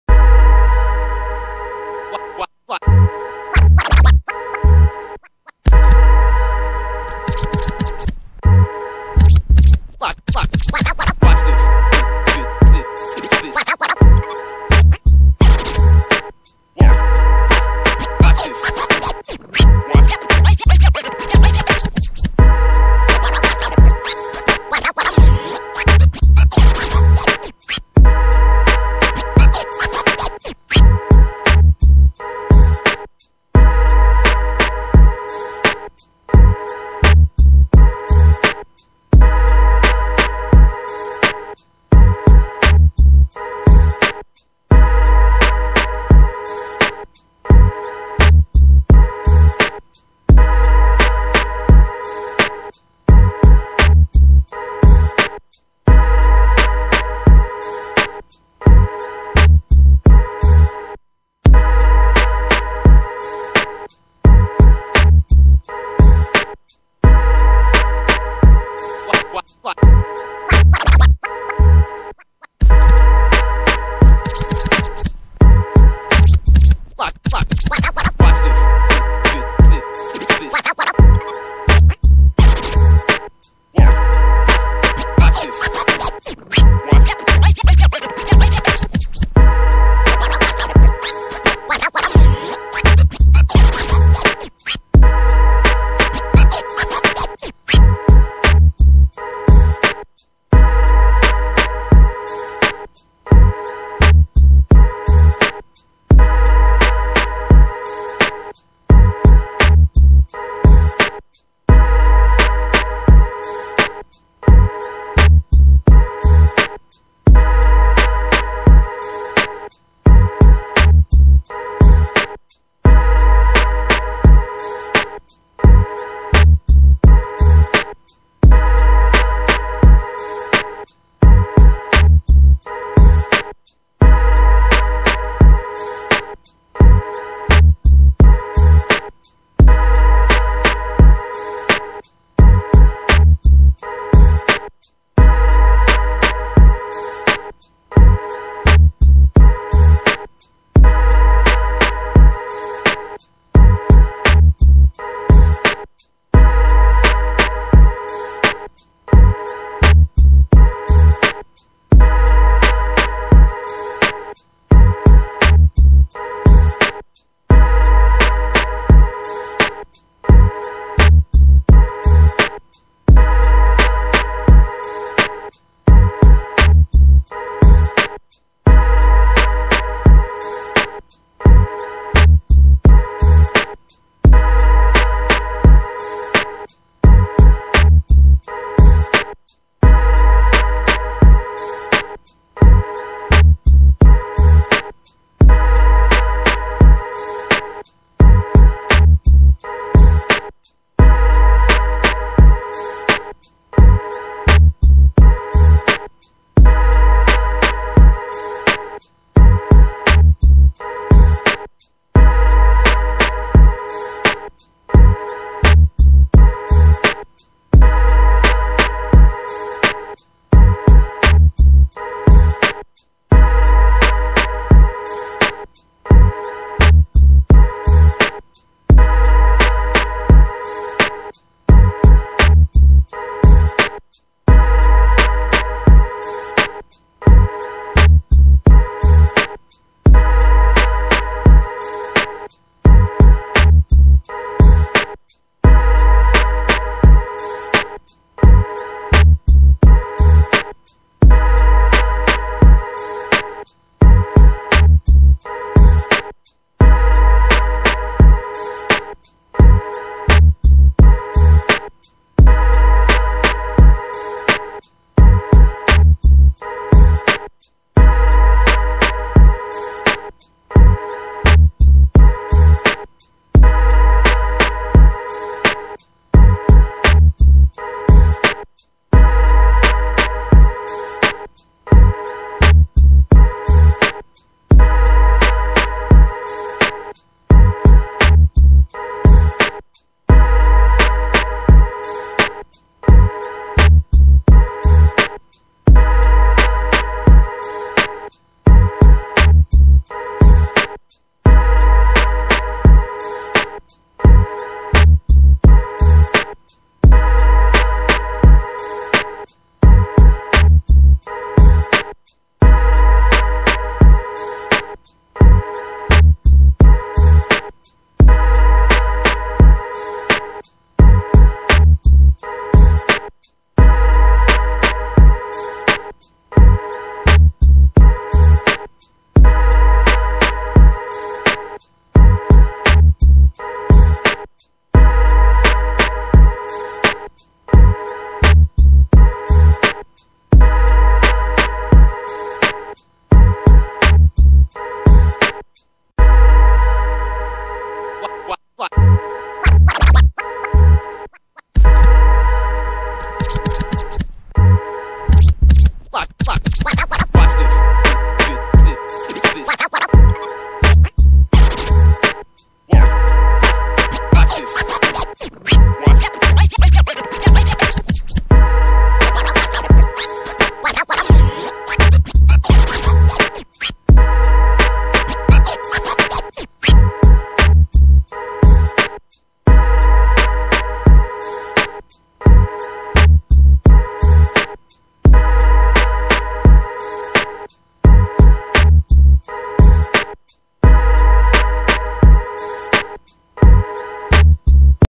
EVIDENCE OF GEOPOLYMER DECOMPOSING AND DETERIORATING
reveals these blocks are not solid quarried stones shaped and stacked. they were made in place with ancient concrete geopolymer. it explains everything!
You will notice these objects have a shell that's peeling and chipping off leaving the inner material exposed proving essentially these are not solid blocks of carved stone. my favorite pic is of brien foerster leaning on the cracked geopolymer like nothing to see here!